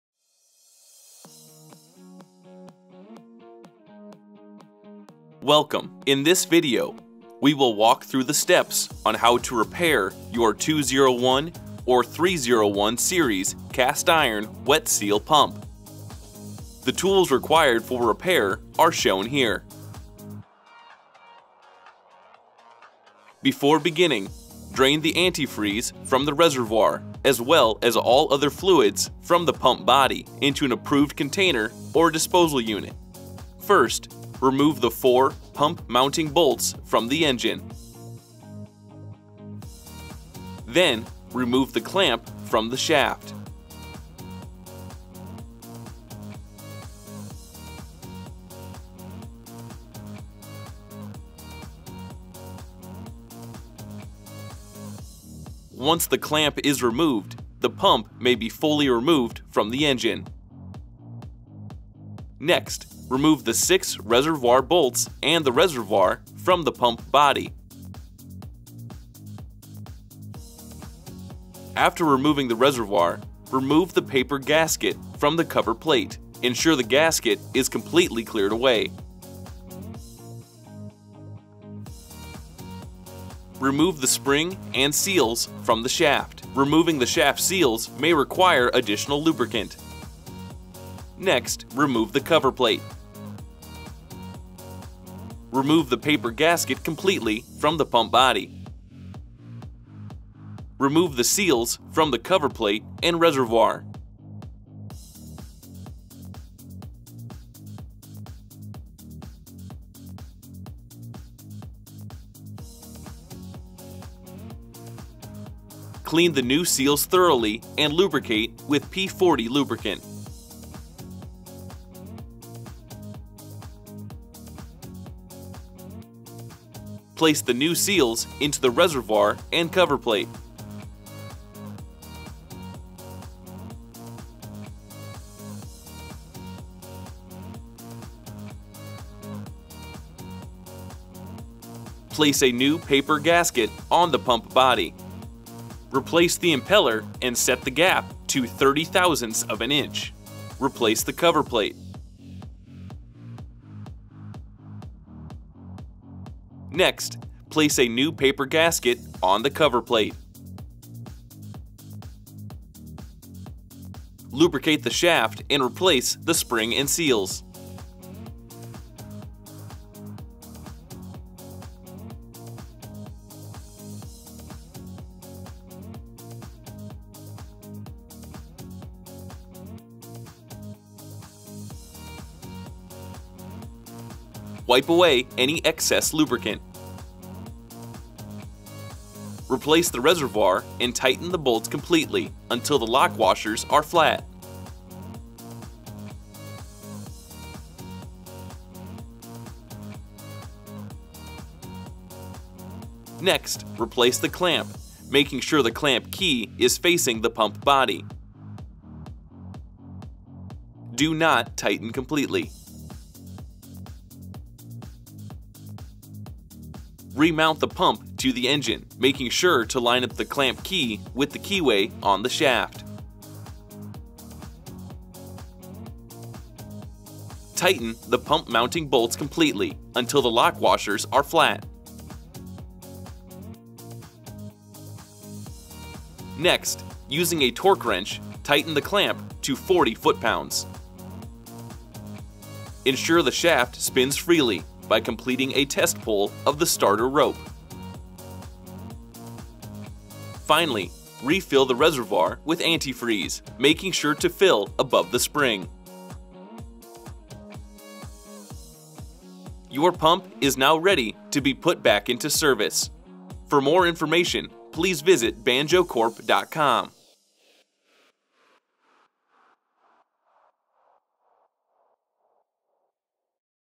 201 & 301 Cast Iron Wet Seal Pump Repair
Learn how to repair your Banjo 201 or 301 cast iron wet seal pump.